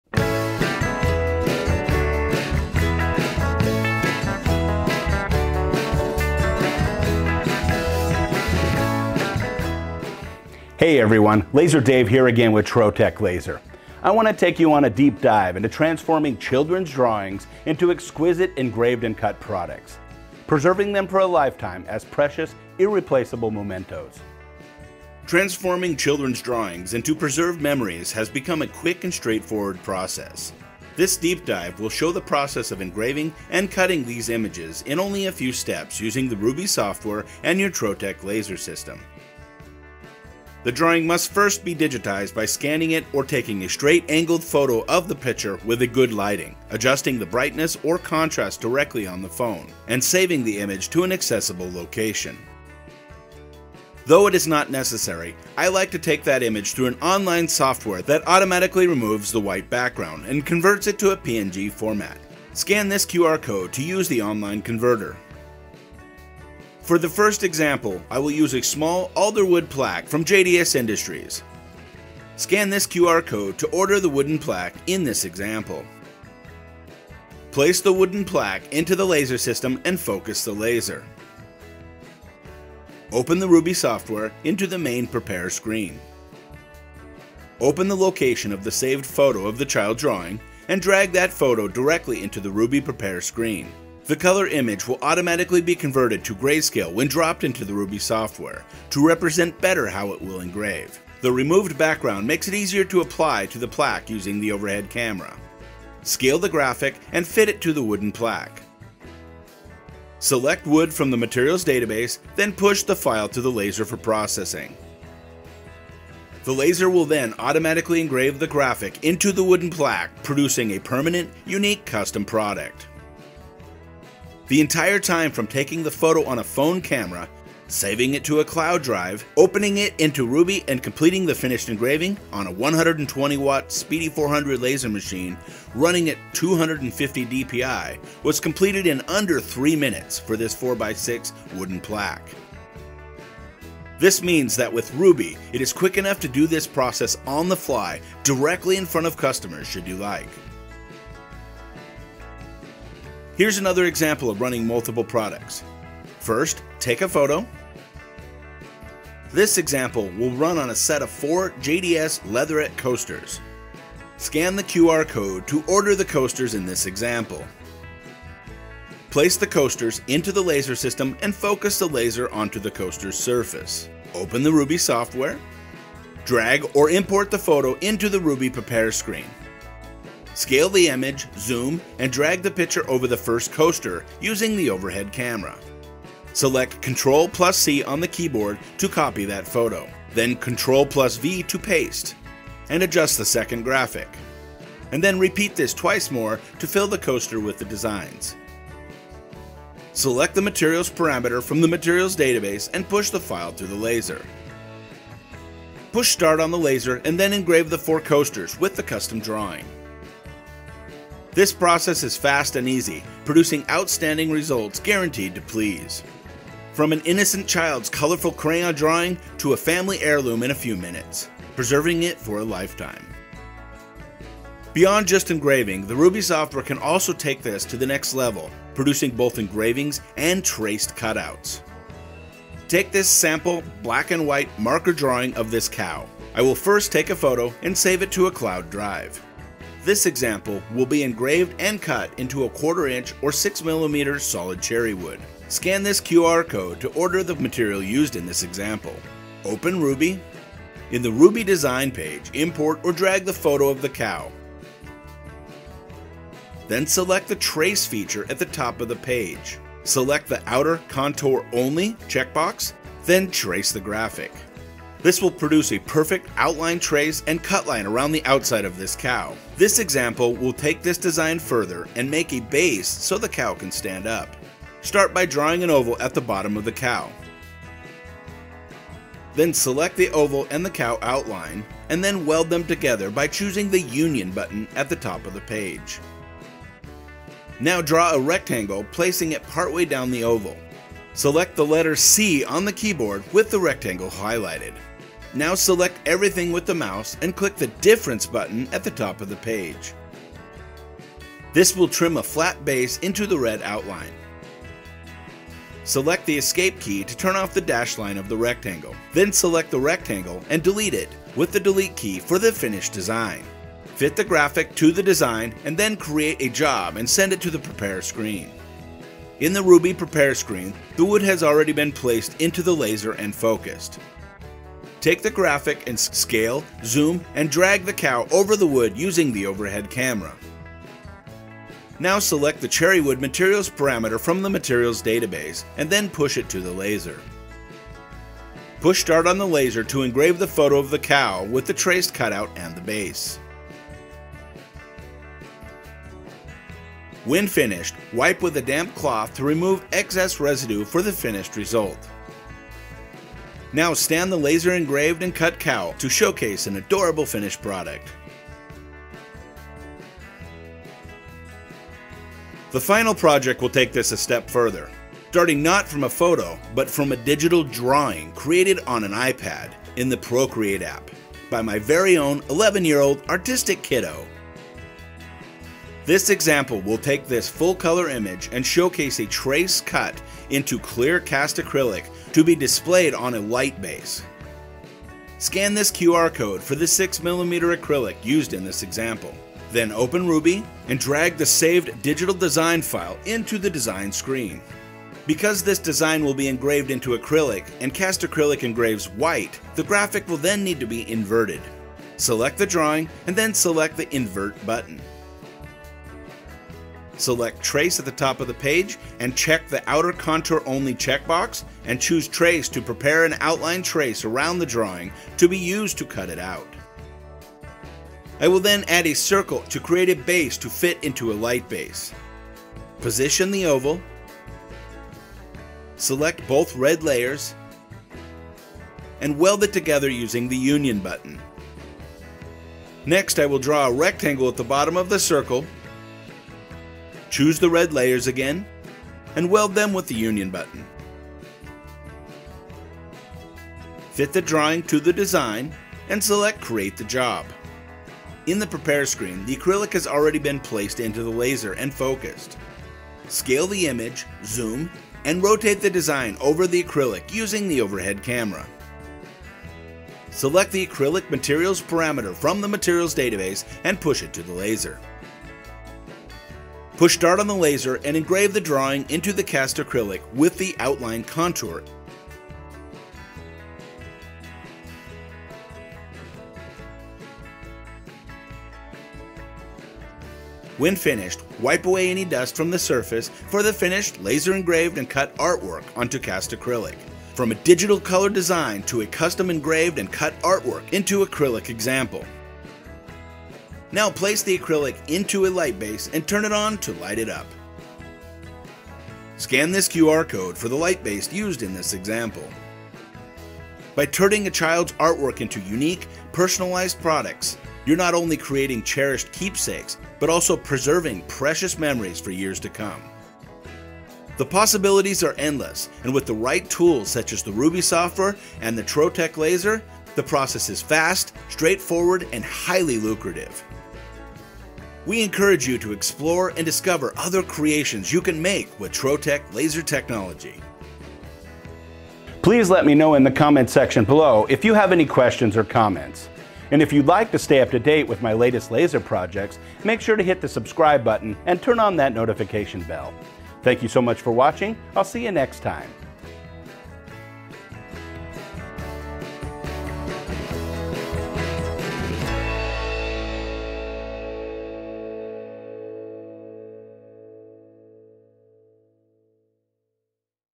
Deep Dive with Laser Dave: Converting a Child's Drawing to an Engraving
We've got another Deep Dive video for you this month! Watch as Lase Dave converts crayon drawings and digital art into engravable designs. Use this process to create your own one-of-a-kind keepsakes.

Be the first to get updates!